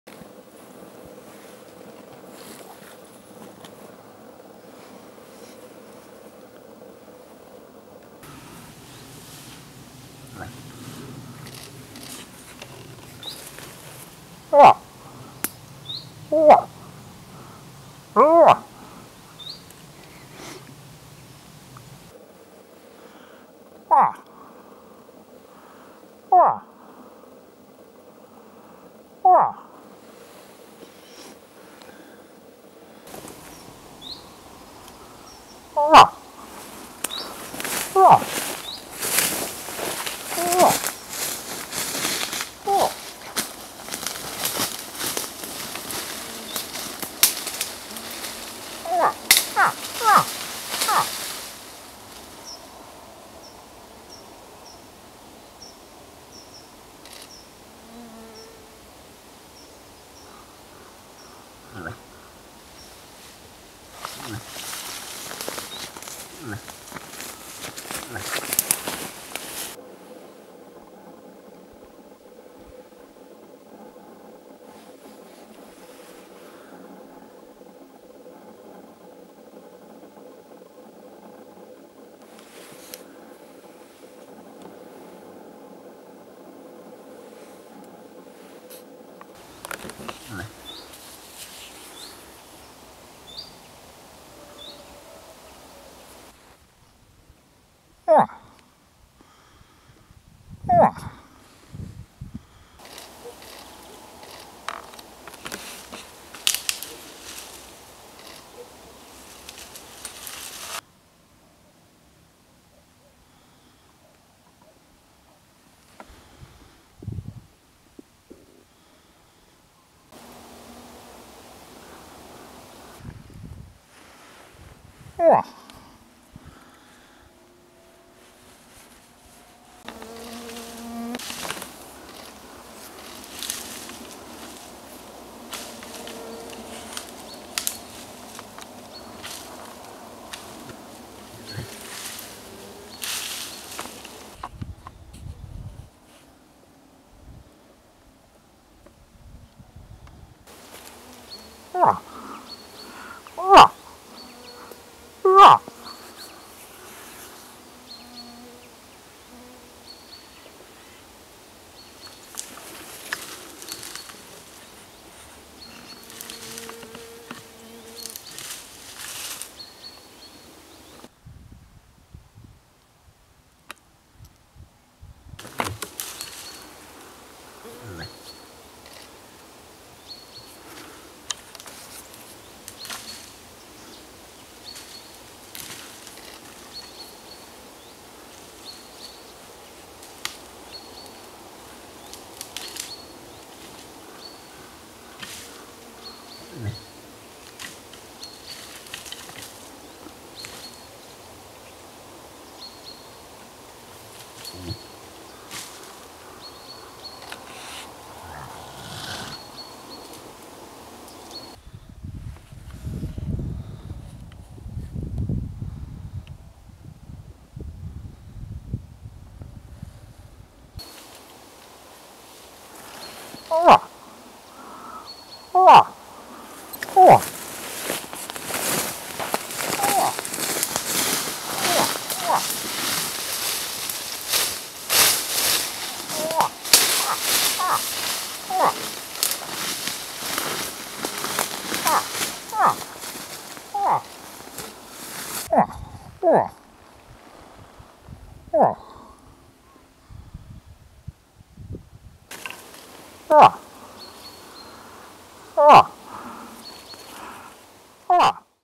Лось во время гона. Киносъемка "на вабу". Elk during the rut.
Голос лося "за кадром" - Виктор Козловский.
Видео - Виктор Козловский.
Voice moose "behind a shot" - Victor Kozlovsky.
Video - Victor Kozlovsky.